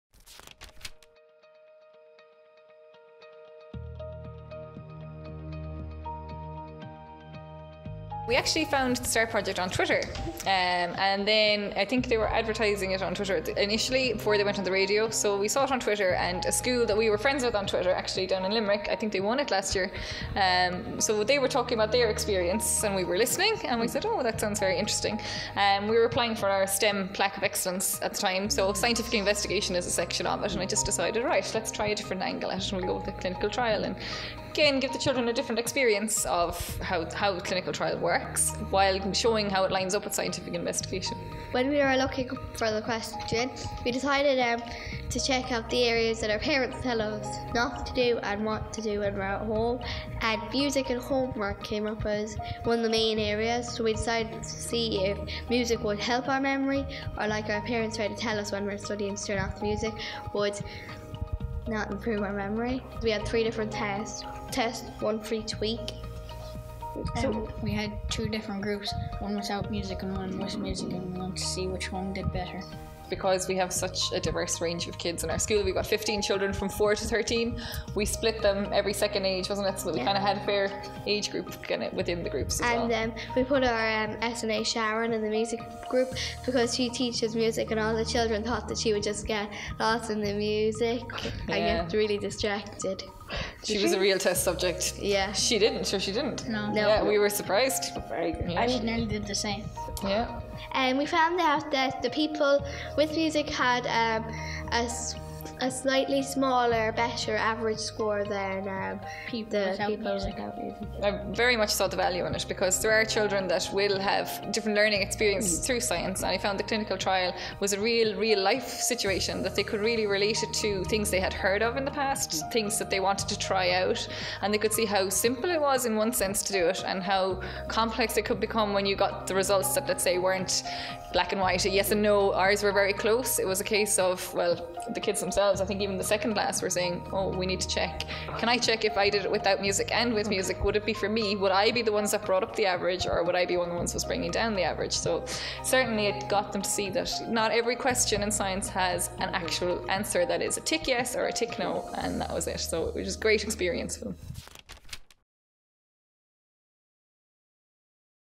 Cloghan's Hill National School 1st Place START Winners 2017
1st Place START Winners share their experience of creating their very own simulated randomised control trial - Does music help improve your memory?